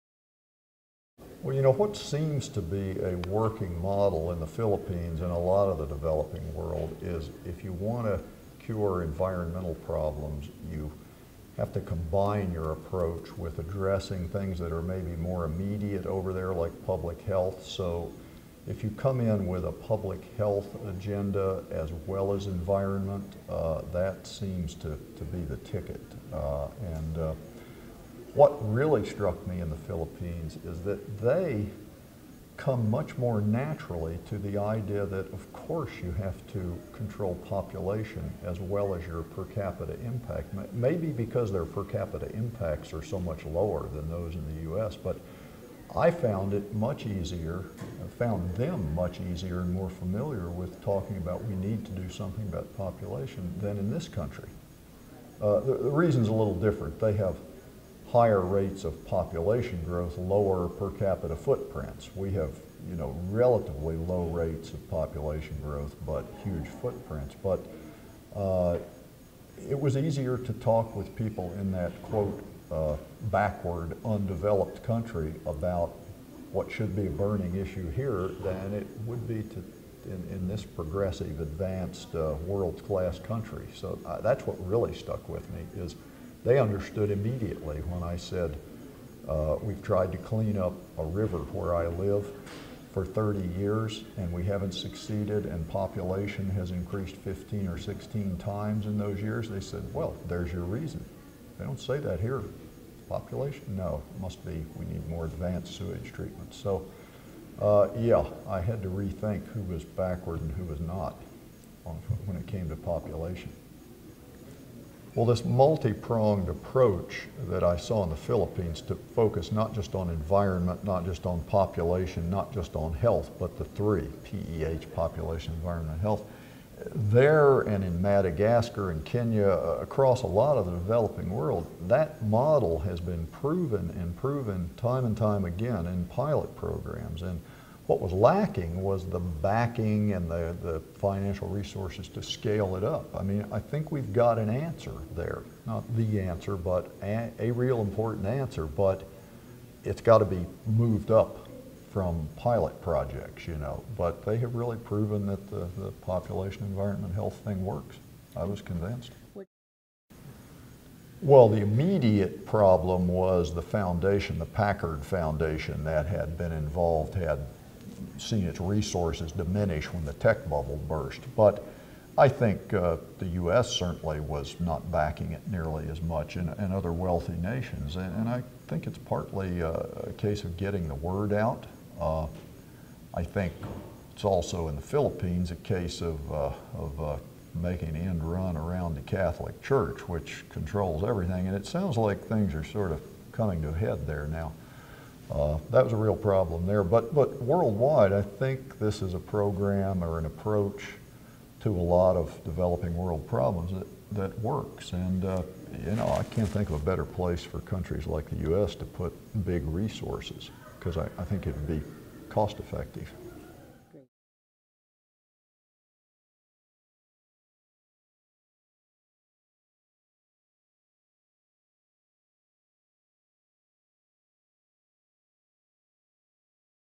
SEJ Conference 2008 - Tom Horton: Environment, Public Health, and Population in the Philippines
In this short video Tom Horton, author and freelance journalist, discusses comprehensive approaches to development including examples from the Philippines, a country that integrates public health, environmental, and population project approaches. This footage was shot at the 2008 Society of Environmental Journalists conference, which took place in Roanoke, VA.